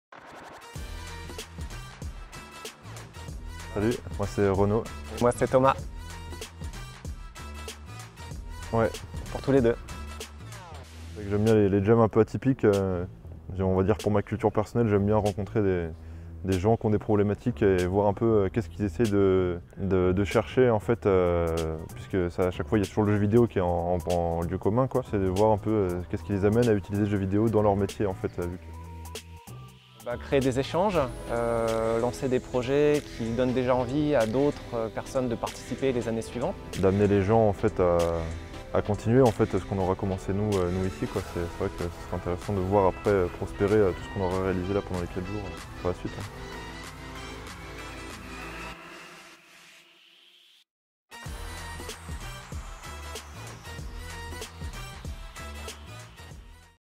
Health Game Jam OpenGeneva 2018 - Developers
Outcomes of game jam which took place on April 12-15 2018 at the University of Geneva and Genevan University Hospitals as part of the OpenGeneva festival.